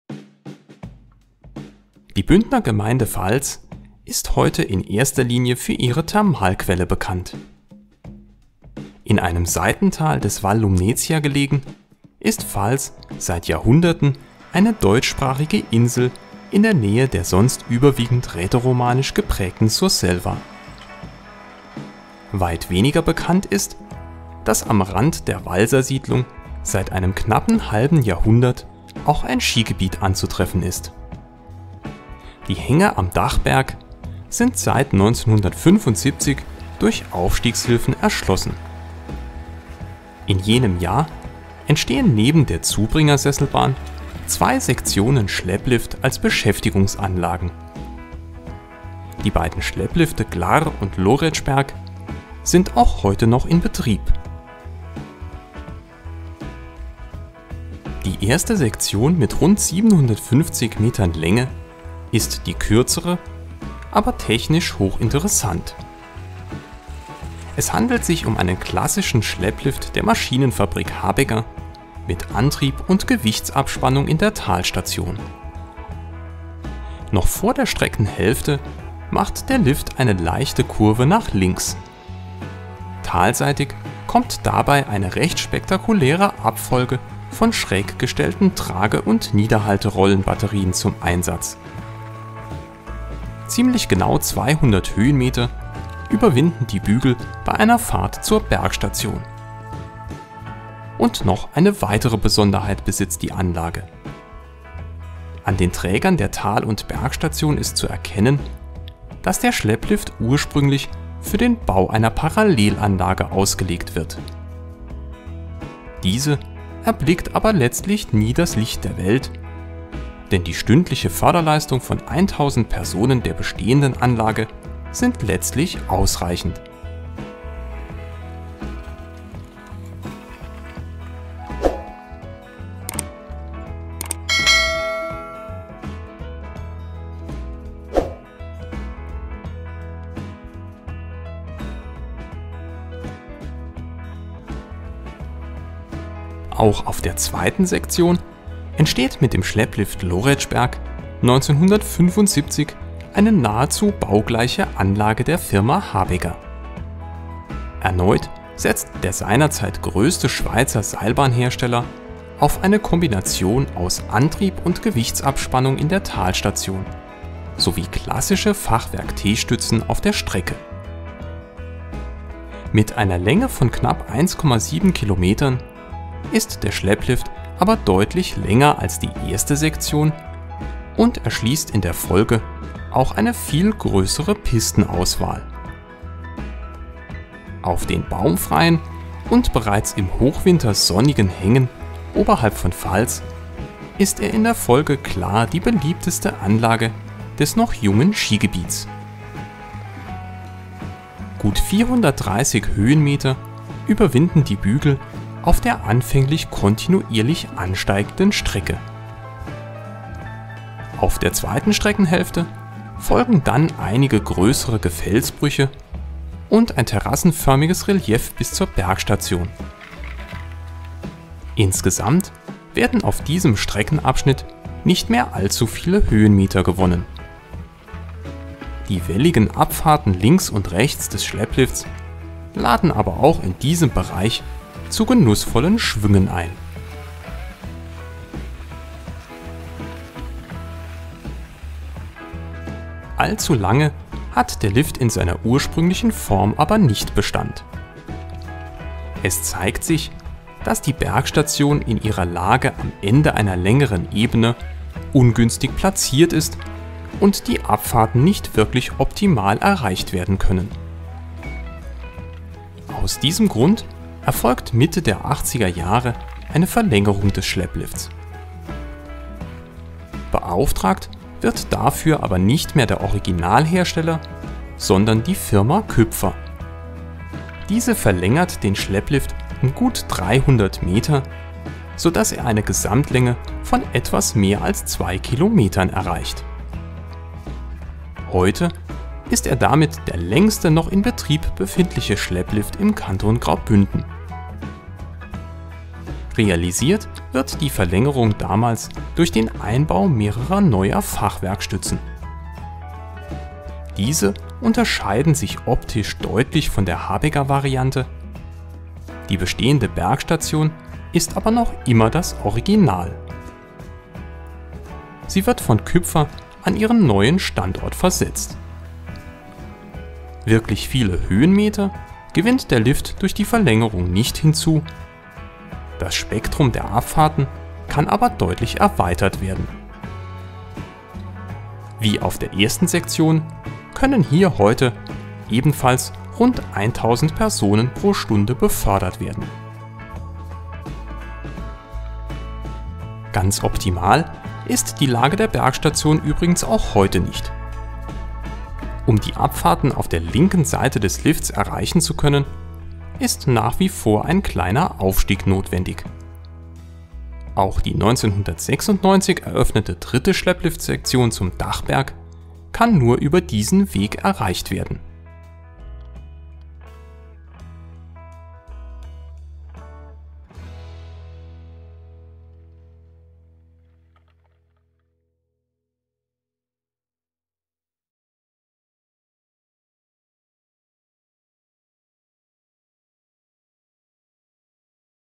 Glarr & Loretschberg | Zwei Schlepplift-Klassiker von Habegger in Vals
Die Bündner Gemeinde Vals ist heute in erster Linie für ihre Thermalquelle bekannt. In einem Seitental des Val Lumnezia gelegen ist Vals seit Jahrhunderten eine deutschsprachige Insel in der Nähe der sonst überwiegend rätoromanisch geprägten Surselva. Weit weniger bekannt ist, dass am Rand der Walsersiedlung seit einem knappen halben Jahrhundert auch ein Skigebiet anzutreffen ist. Die Hänge am Dachberg sind seit 1975 durch Aufstiegshilfen erschlossen. In jenem Jahr entstehen neben der Zubringer-Sesselbahn zwei Sektionen Schlepplift als Beschäftigungsanlagen. Die beiden Schlepplifte Glarr und Loretschberg sind auch heute noch in Betrieb.

Produktion: Felix Gross
Aufnahme: 23. Februar 2021